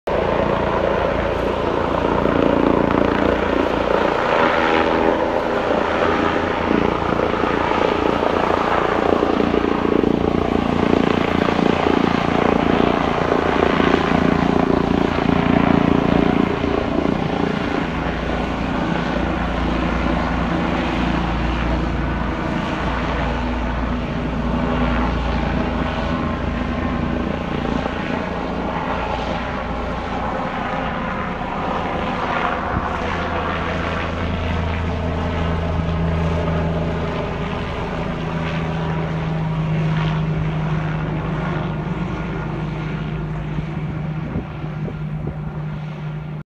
G-KSSC Taking off from the Faversham cricket grounds 12/04/20
This video was taken from my front garden. I was not out on any form of walk etc.